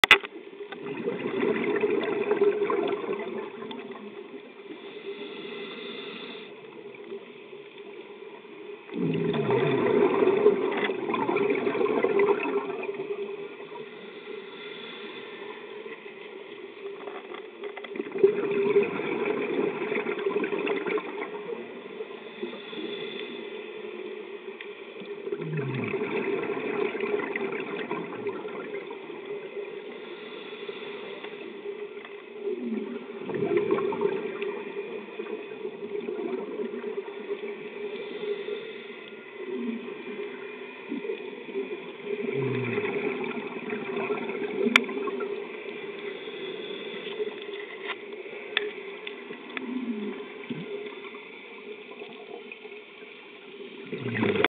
Potato Bass cleaning station
I filmed this guy at Sodwana bay, South Africa. Cleaner fish make their simbiotic living by cleaning the parasites off bigger fish's gills and bodies. The potato bass gets a free exfoliation and the cleaner wrasse gets a free meal, everyone's happy! By Burd Cordier